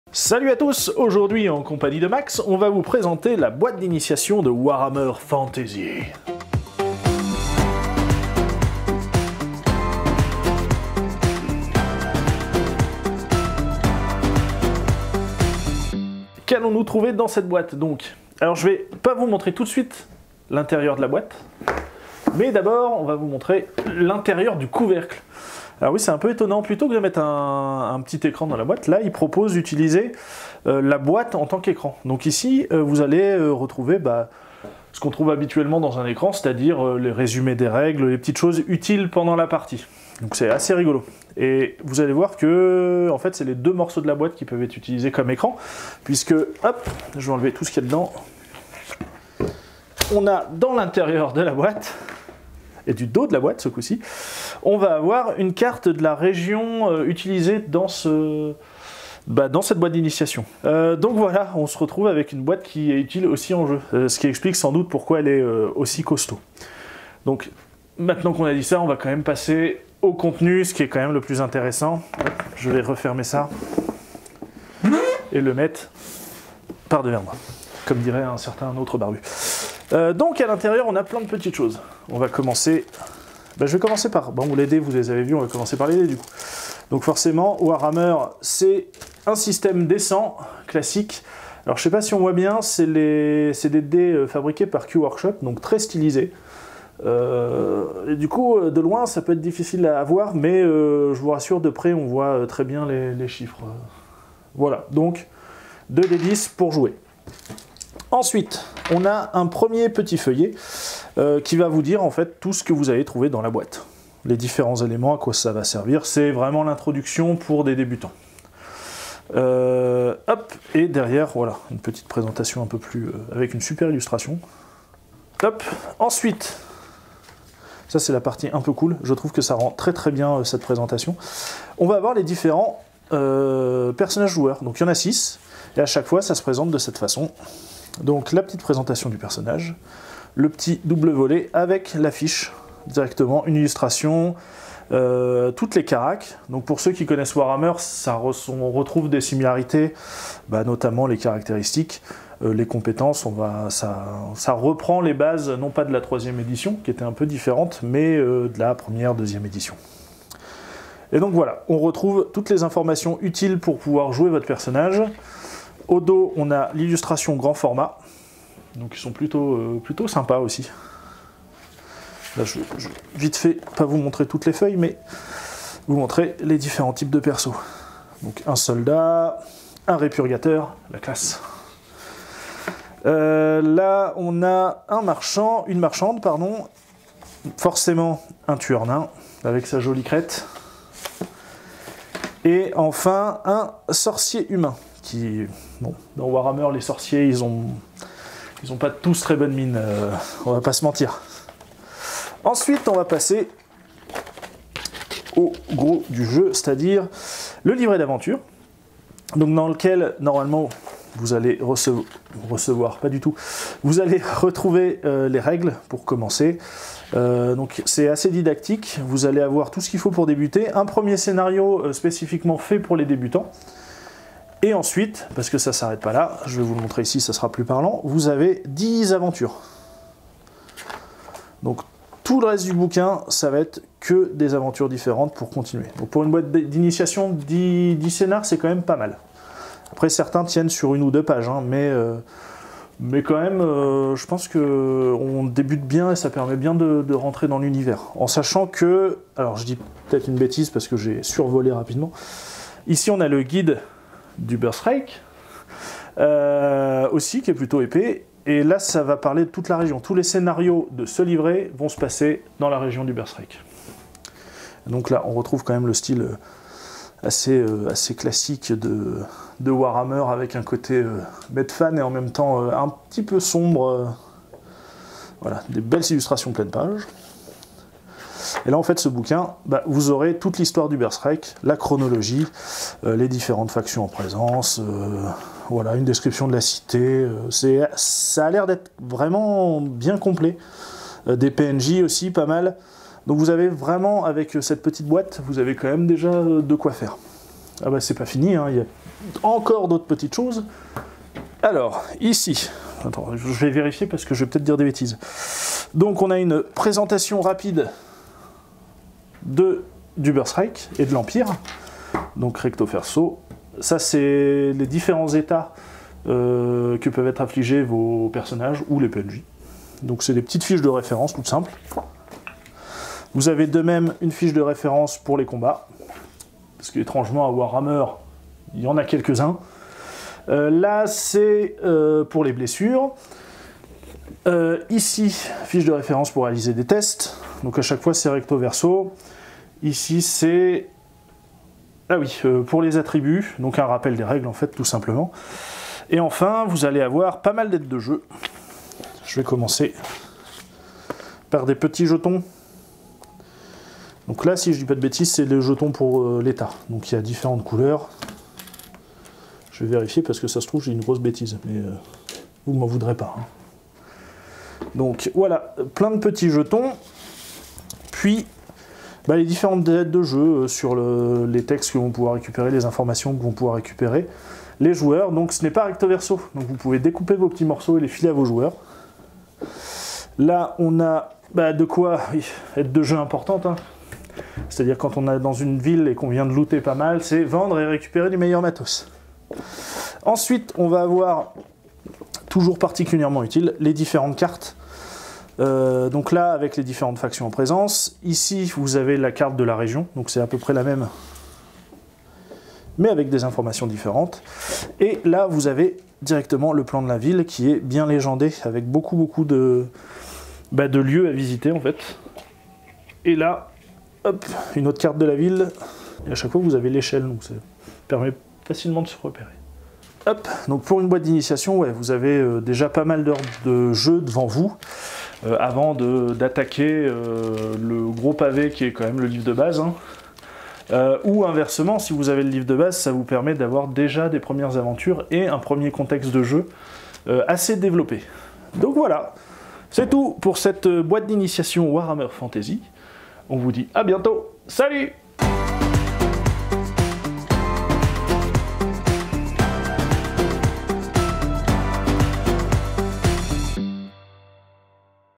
Warhammer Fantasy - Boite d'Initiation - OZB
Vous souhaitez vous plonger ou vous replonger dans l'univers riche et varié de Warhammer ?
Découvrez la sympathique boite d'initiation que propose Khaos Project, qui contient tout ce qu'il faut pour se lancer dans l'aventure.